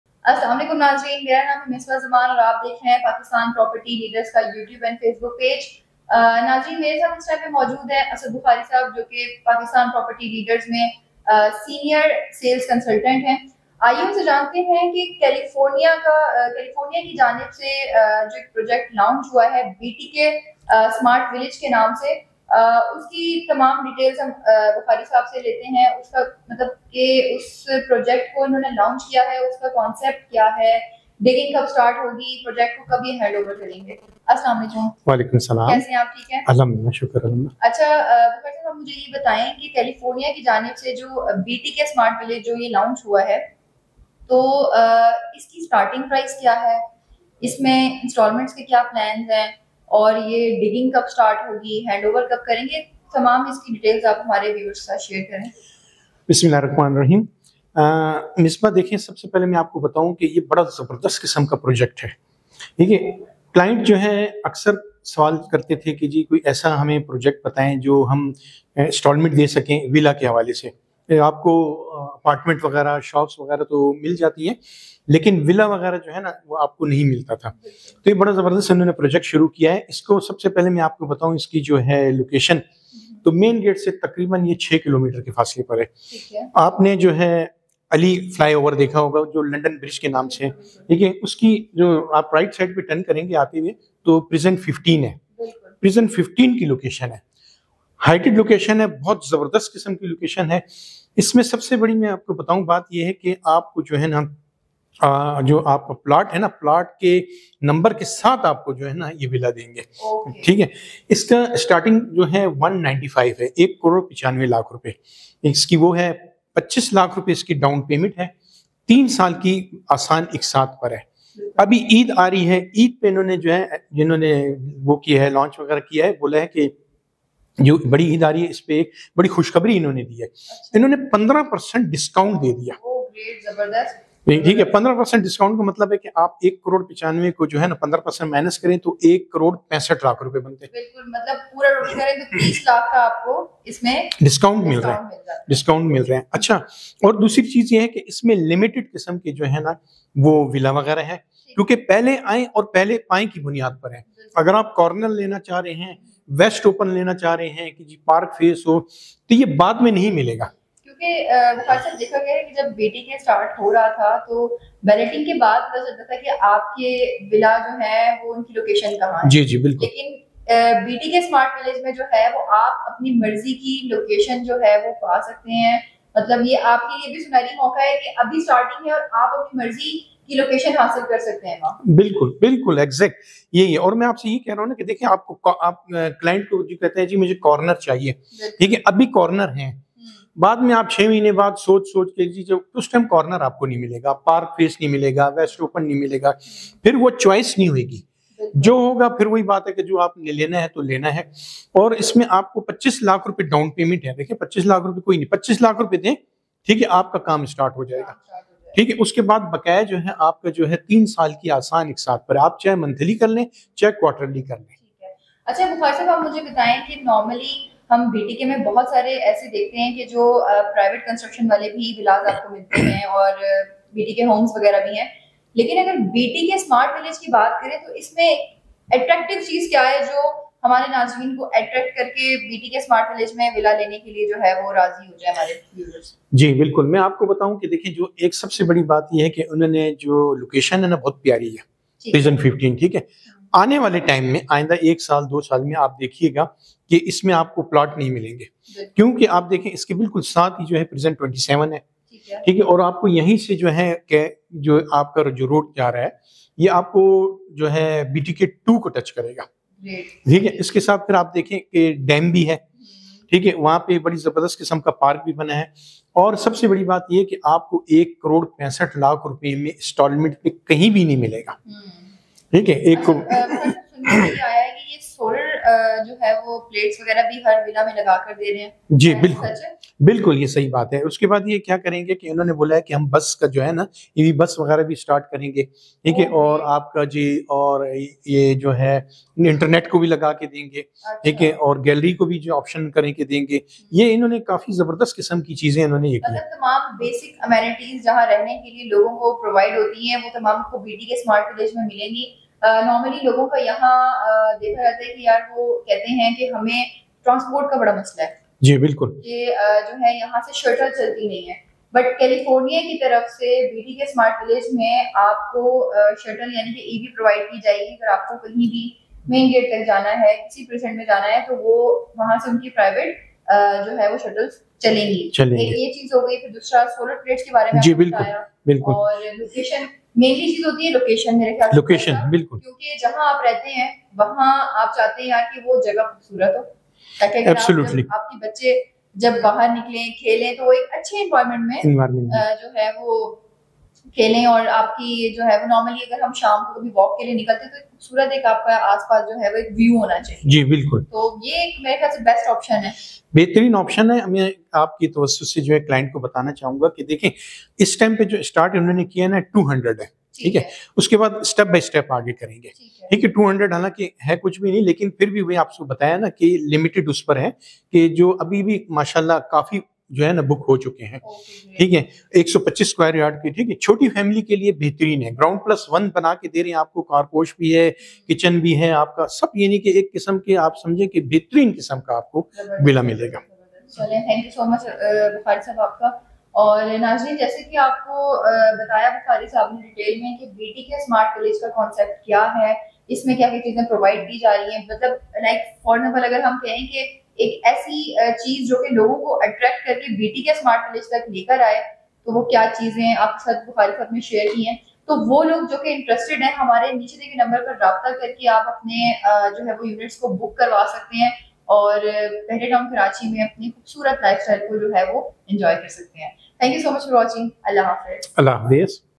BTK Smart Village| Detailed Information Of New Deal| Payment Plan
BTK Smart Village| Detailed Information Of New Deal| Payment Plan btksmartvillage 125sqyardvillas bahriatownkarachi bahriatown #2024

BTK Smart Village
125 SQ Yard Villas On Installments
3 Bedroom
3 Bathroom
2 Kitchen
Drawing Room
TV Lounge.
Precinct 15
Big Discount Offer
Payment Plan
Down Payment 2500,000
On Possession 1550,000
And Remaining Amount 125,25,000 in 3 Years Easy Instalment plan.

Times Of Bahria:
/ pakistanpropertyleaders